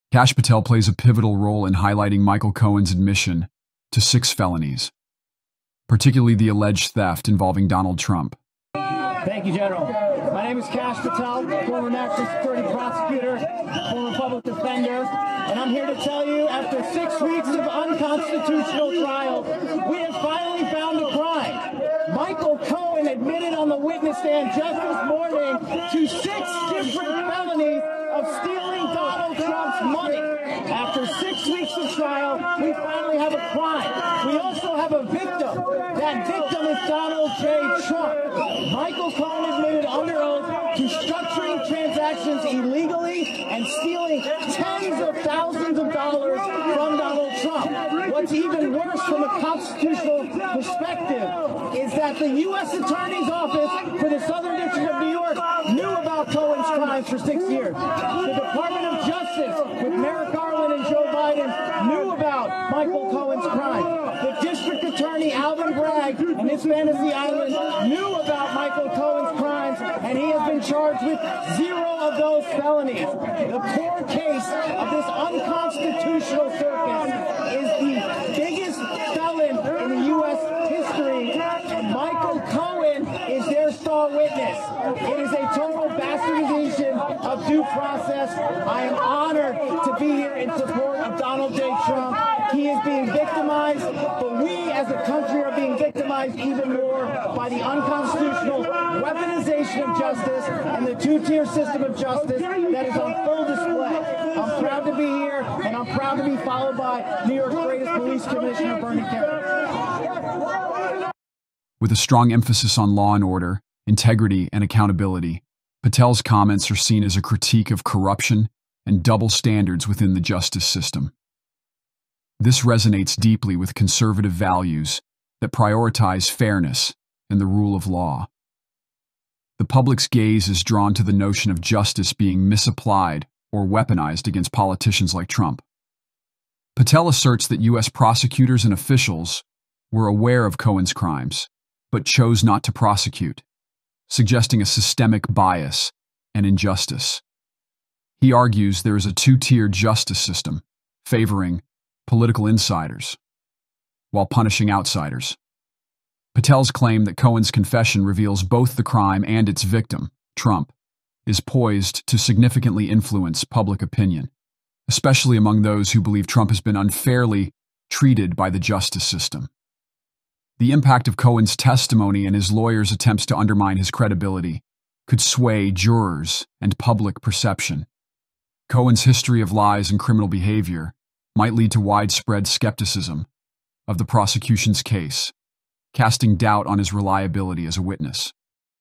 Kash Patel, Michael Cohen admitted on the witness stand to 6 different felonies
Kash Patel shocks the court by revealing that Michael Cohen admitted to six different felonies on the witness stand. In this explosive testimony, Patel details how Cohen confessed to structuring illegal transactions and stealing tens of thousands of dollars from Donald Trump. This revelation highlights the alleged mishandling and cover-up by the U.S. Attorney's Office for the Southern District of New York and the Department of Justice. The video delves into the constitutional implications of these actions and the broader context of the case, showcasing the intense legal and political drama unfolding. Don't miss this crucial update on one of the most significant trials in recent history.